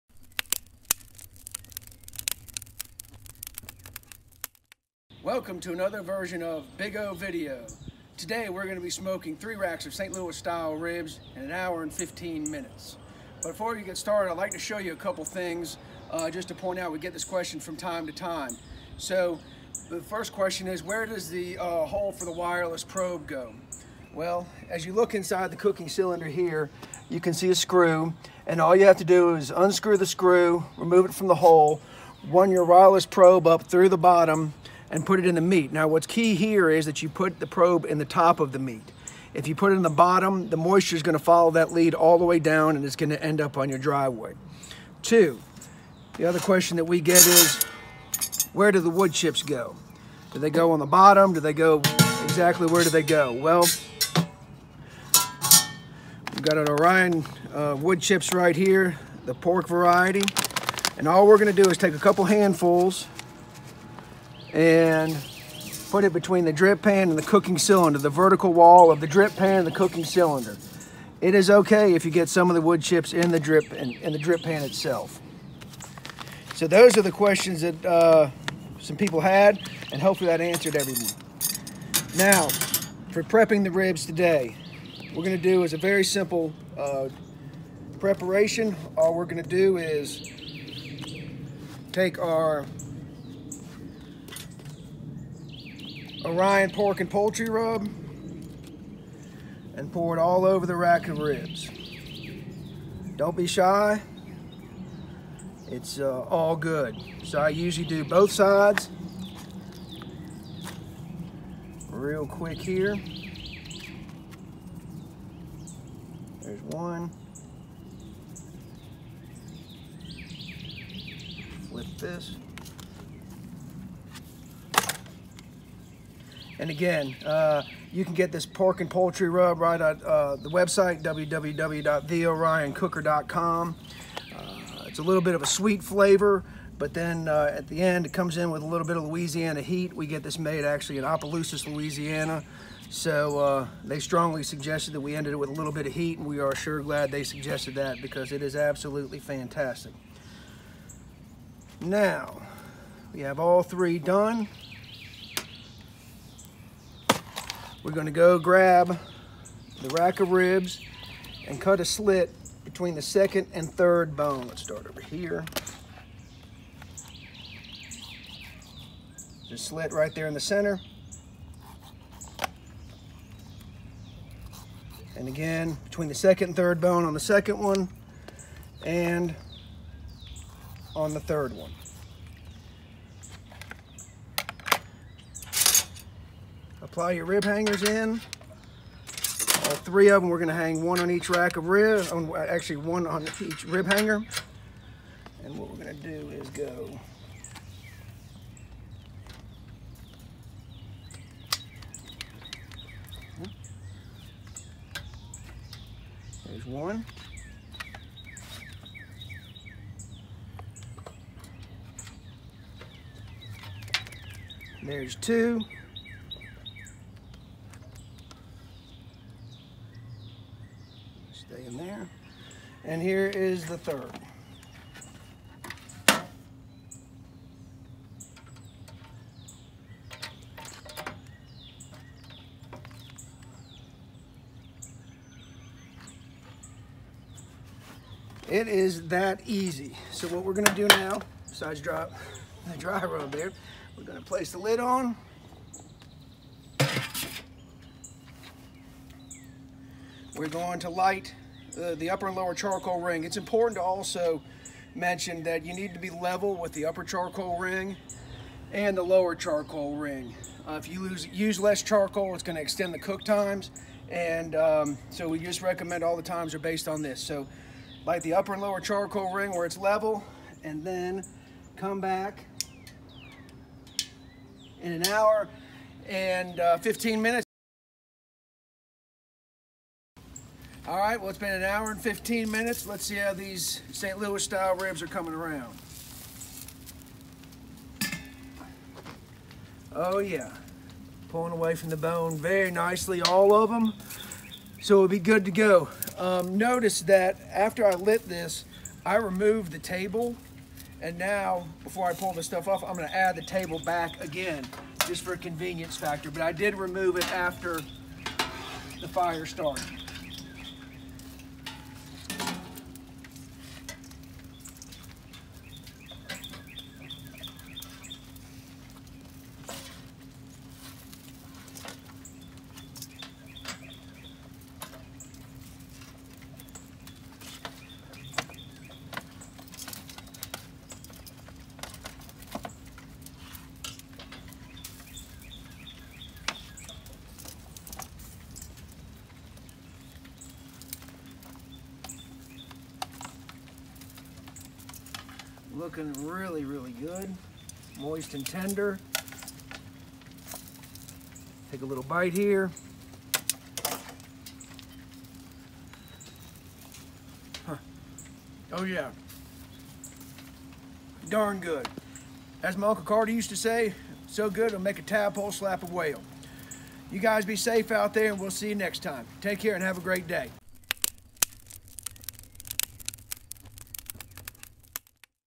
How to Smoke 3 Full Racks of St. Louis Style Ribs in ONLY 1 hour & 15 minutes with The Orion Cooker!
Christian Fitzgerald , shows you how to cook St. Louis Style Smoked Ribs using The Orion Cooker and walks you through the process of smoking and seasoning 3 full racks of them in only 1 hour and 15 minutes.